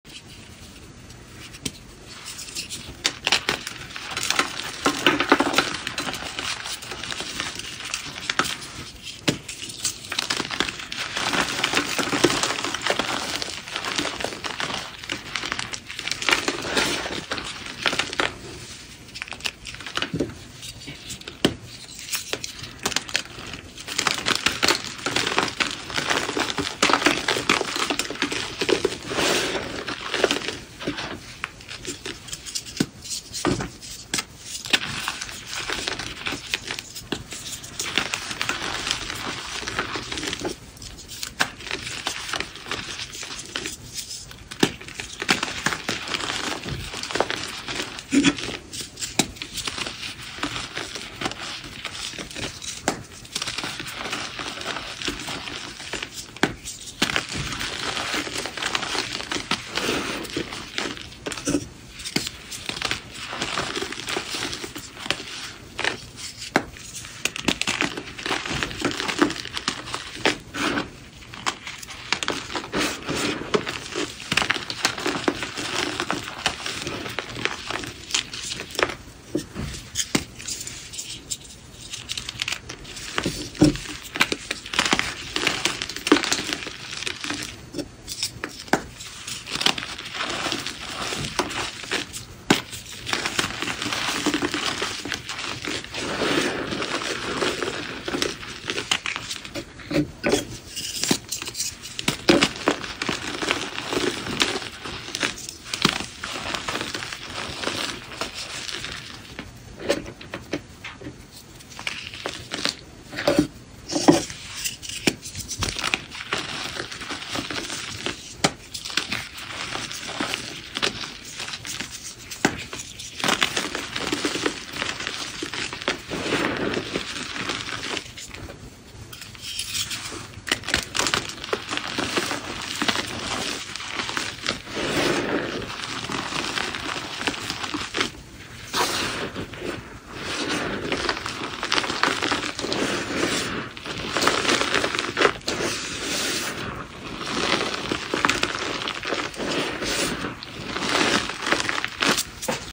Crushing @alzasmr reforms!! - dusty, crunchy, powdery
WELCOME TO MY 300th YOUTUBE VIDEO 🎉 FINALLY bought some reforms from @alzasmr ! This is half of the $30 mystery box and the value for it was so worth it.

These reforms were crunchier than I thought they would be, but the sound is so divine!! I’m still learning how to crush with press on nails on so that they don’t break off so my crushing style with harder chalk is a little weird now LOL. I’d say these were a medium texture, crunchier on the outside than on the inside for sure.

They are very dusty, probably due to the fabric softener in them. They’re not a pure GC mix, which is perfect because when you buy reforms, you want that ASMRtists special touch that you usually can’t get from plain jane reforms. I also LOVE the color scheme. The only downside is the scent is pretty strong, but I know some people love that based on the poll I did on IG a few weeks ago.